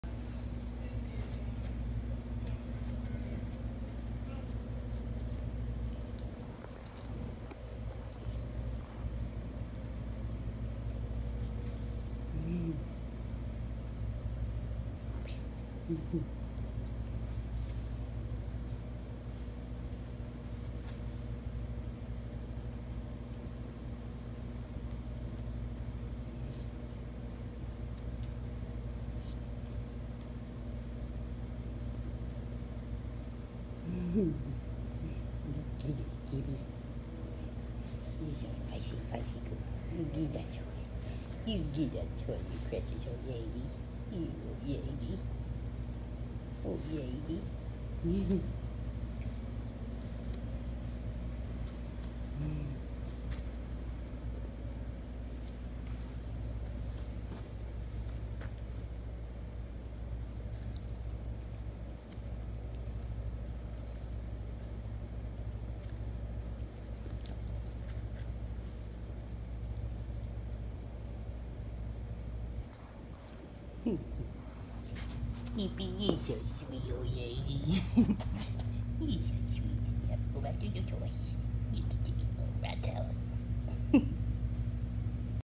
McCrory’s Fancy Ferrets
My old lady skippy playing with her toy. She has adrenal disease but gets her DES implant, it treats the symptoms but did not grow the hair back. It’s okay, she is happy and it’s just hair. Lol 💜 She just turned 9!!!